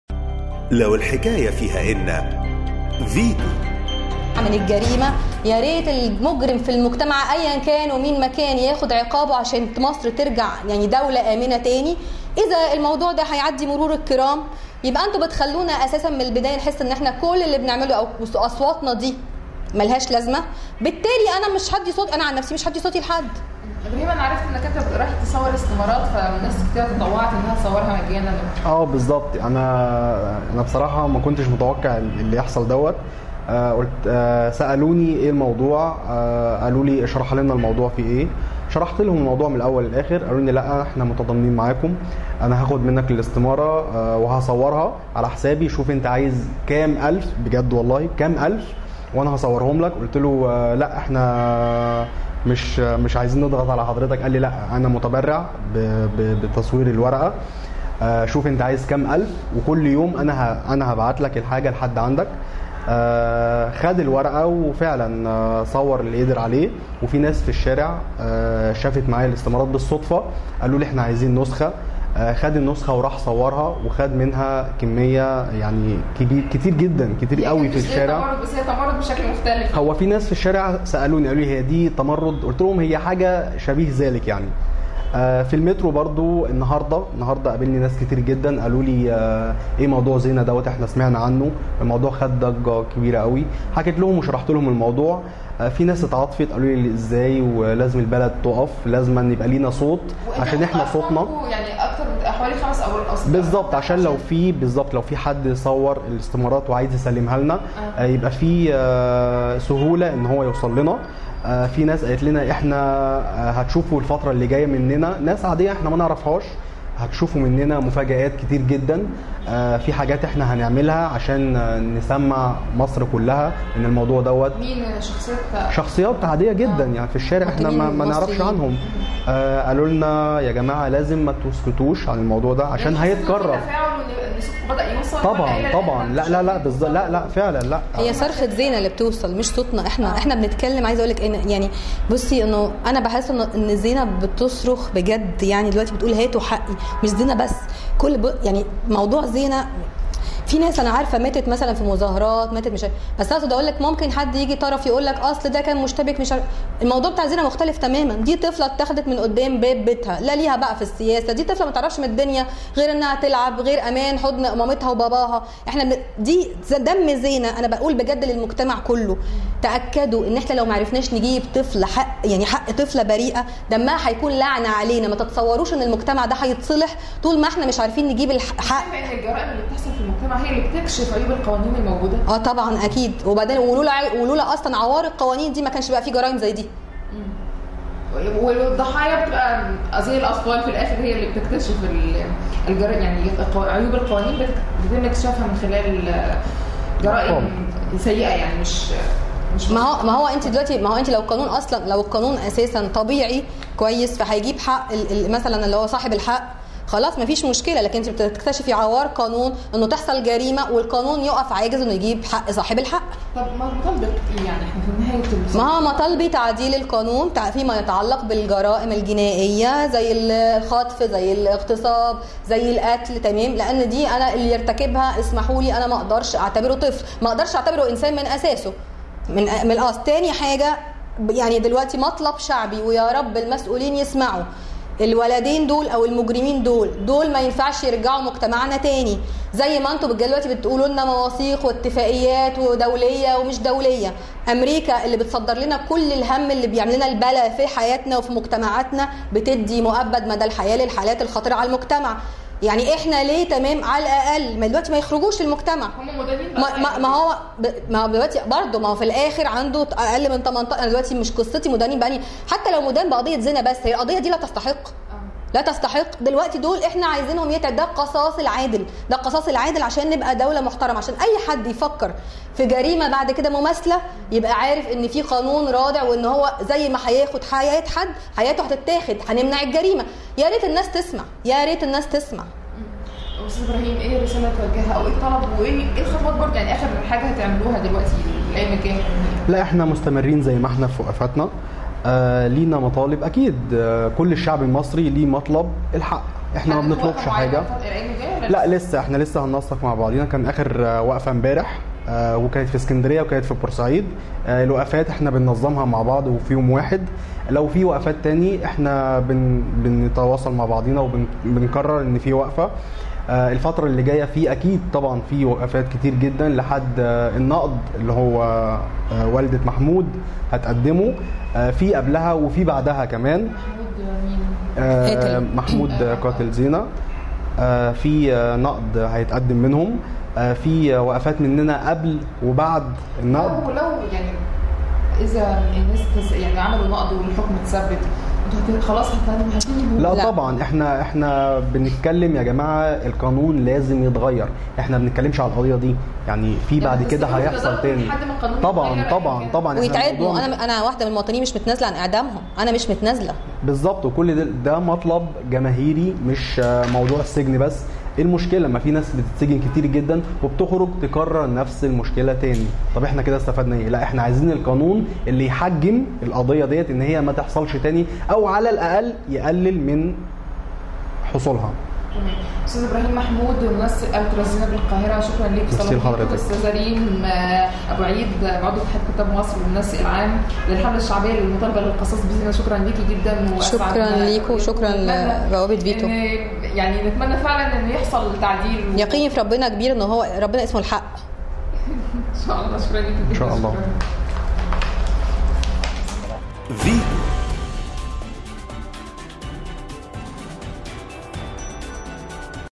منسقة حملة "القصاص لزينة": لن نهدأ حتى تعديل قانون الطفل ج1
أكد ابرهيم محمود منسق حملة ألتراس زينة بالقاهرة أن الحملة بدأ تشكيلها قبل الحكم في قضية الطفلة زينة بعدما وجدوا أن نفس المشكلة يتم تكرارها ولا يوجد أي رد فعل تجاهها،ونوه أن اختيارهم اسم الألتراس للحملة يرجع إلى أن الاسم قادر على الحشد السريع، ورغم أنه يعني شدة التعصب لكن ليس عنفا، وإنما المطالبة بالحق.